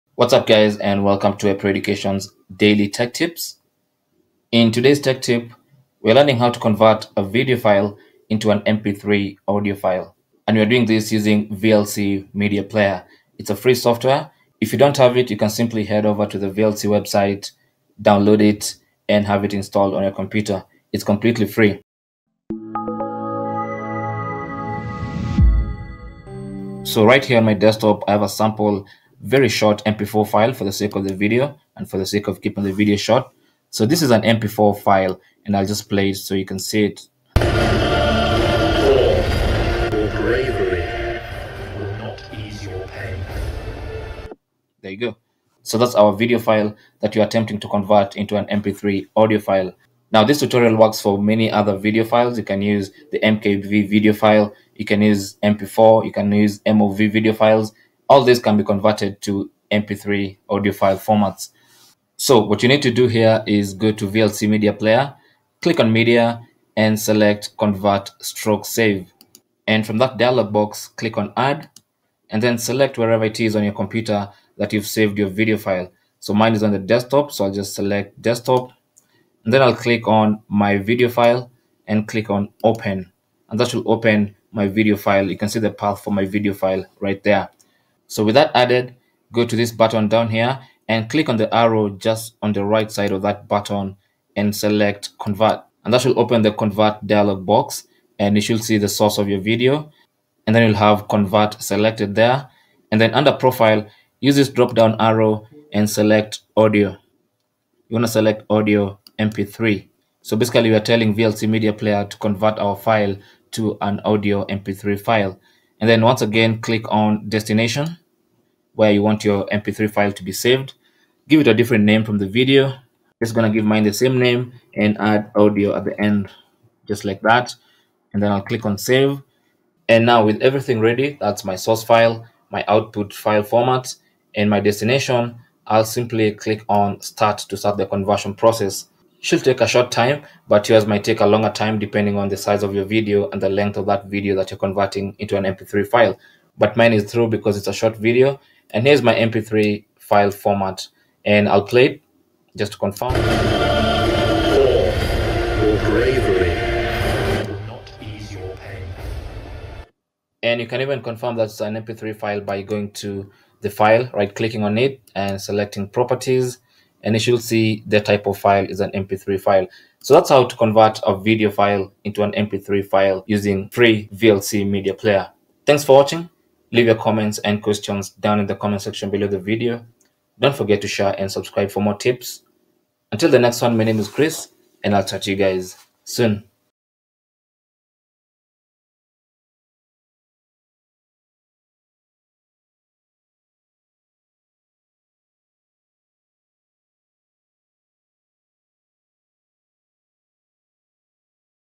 How To Convert Video To MP3 Audio Using VLC
Learn how to convert a video to an MP£ audio file using VLC media player;  an open source program.

How To Convert Video To MP3:
Install VLC media player
Add the video you want to convert to mp3
Choose destination file type and folder
Convert.

Use VLC Media player as a Video to MP3 converter. This is one of the many things VLC can do besides playing videos and mp3 files.

This program can convert any video file it can play to either MP3 or any other audio file format.